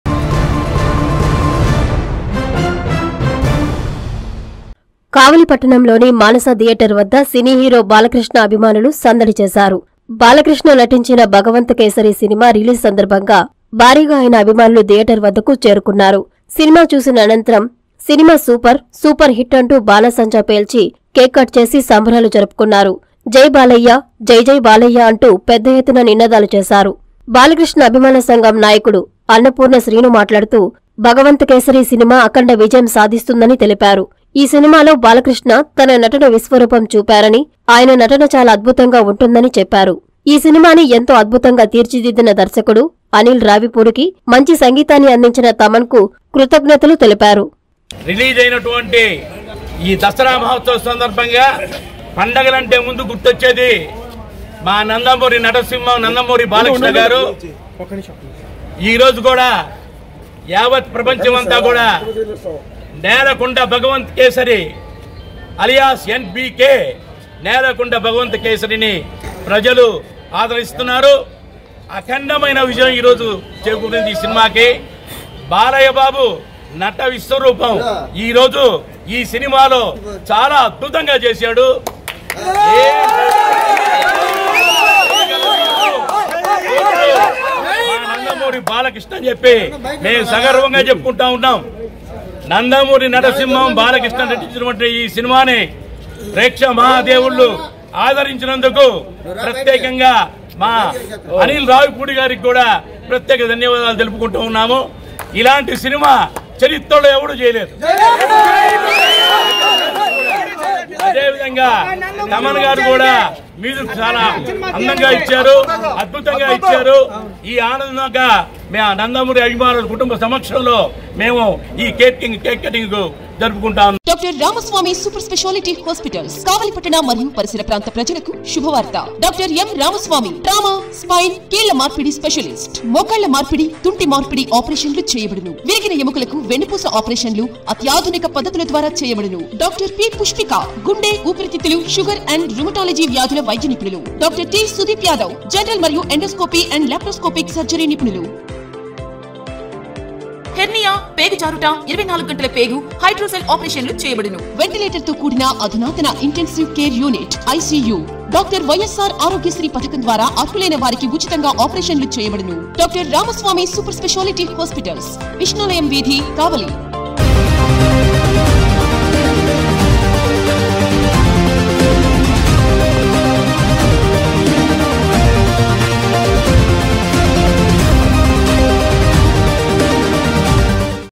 భగవంత్ కేసరి అఖండ విజయం | Balakrishna fans Hungama | Bhagavanth Kesari Movie | Kavali
- కావలిలో సినీ హీరో బాలకృష్ణ అభిమానుల సందడి..
- మానస థియేటర్ వద్ద భగవంత్ కేసరి సినిమా చూసి సంబరాలు